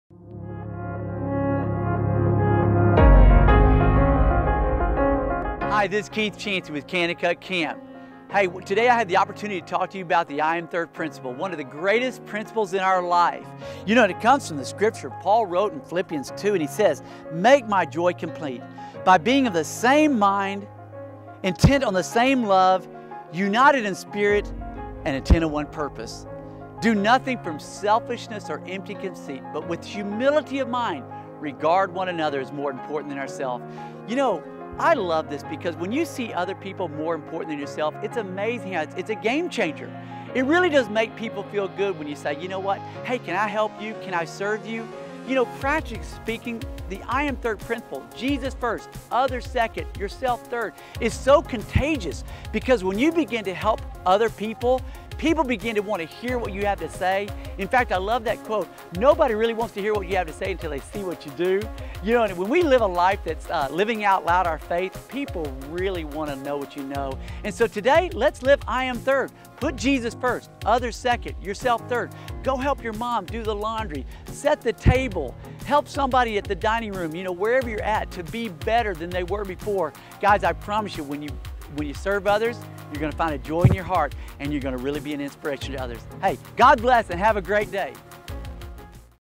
Keith Chancey - Living I'm Third
Here at Kanakuk, we live by the I'm Third principle; God first others second and I'm Third. Living out the I'm Third lifestyle is the key to loving Christ and others well! Keith Chancey, K-Seven Director, shares how Paul's words on serving others from Phillipians 2 can transform our relationships with others.